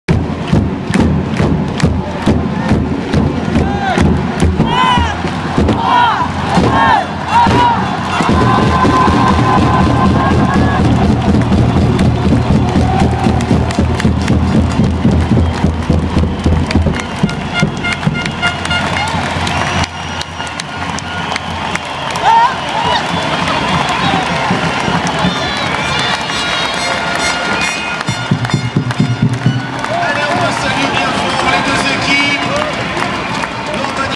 Finale hockey sur glace Boxers de Bordeaux / Hormadi d'Anglet
Match 3 de la finale de D1 de hockey sur glace. Boxers de Bordeaux contre l'Hormadi d'Anglet. Match a la patinoire Meriadeck a Bordeaux. Victoire des Boxers 7-4.